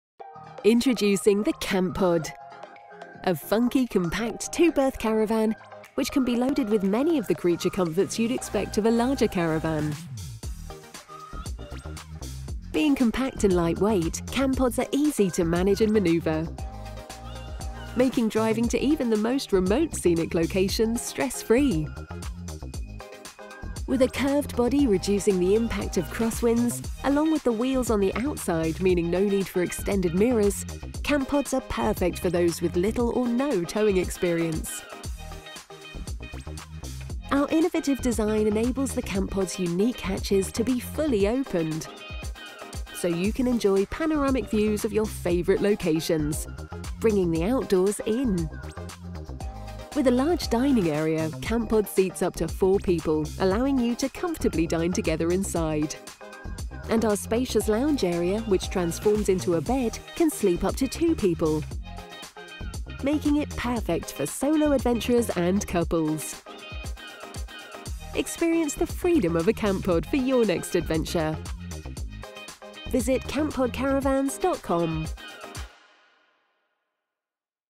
Introducing Campods - the Ultimate Funky lightweightsmallcaravan!
Campod Promotional video - Introducing our brand new Funky Lightweight Small Caravan for 2022!

Campod Caravans - the home of Funky Lightweight Small Caravans!

Looking for a Lightweight Caravan that you can be proud to tow?  Breaking the norm of traditional caravans, Campods are Funky, Retro & Modern!

Chocked full of many of the creature comforts you would expect to find in a larger caravan, Campods offer many options as standard + a variety of options for you to go completely off grid..

Narrow enough that you do not need extended mirrors and with all models fully braked, Campods are very easy to tow with models starting at 800KG.  Tow with a small to medium sized car no problem!

Our curved design helps alleviate those fearful cross winds and with a low centre of gravity you will enjoy a pleasant towing experience.

Make your Campod truly 1 of a kind & a personal statement - Custom colours & graphic options available!

Full viewing facilities available from our showroom in Alvington, South Gloucestershire.

Other useful links in terms of what license you may need, your Car's towing capacity & Finance can be found here:

- What can I tow with my license?
- What weight will my car tow?